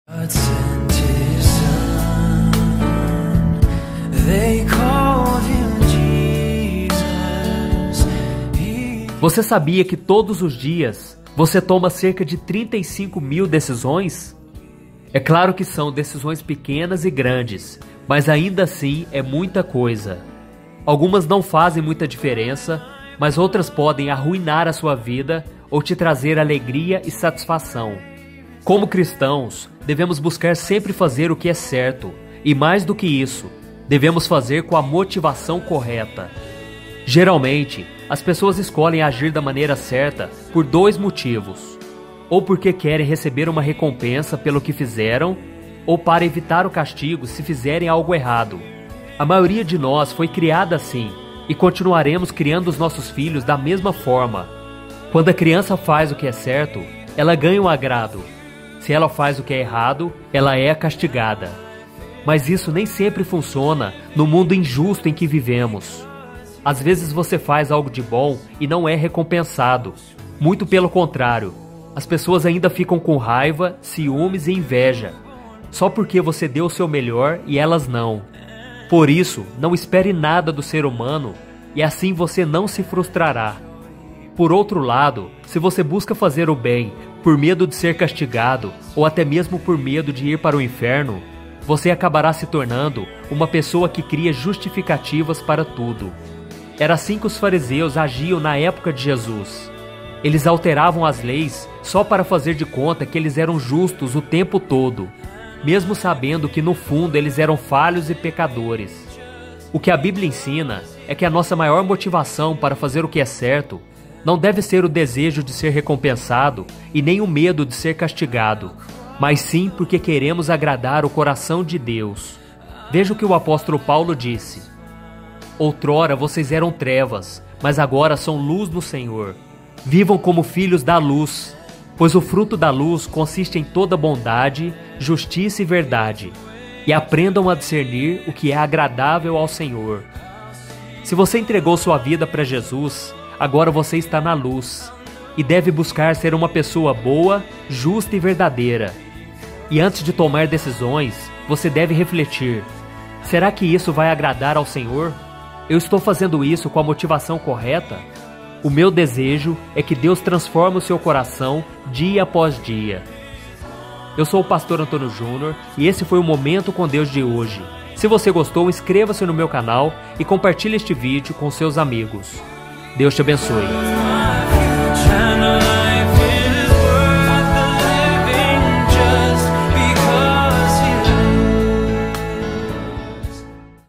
NAS SUAS DECISÕES, VOCÊ BUSCA AGRADAR A DEUS? - Momento com Deus
Analise se você tem buscado agradar a Deus nas suas decisões. Nós cristãos, devemos fazer o que é certo, não porque queremos ser recompensados ou porque temos medo de ser castigados. Devemos tomar as decisões pensando em agradar a Deus!

📕 Adquira os livros do Pastor Antonio Junior com mensagens de fé.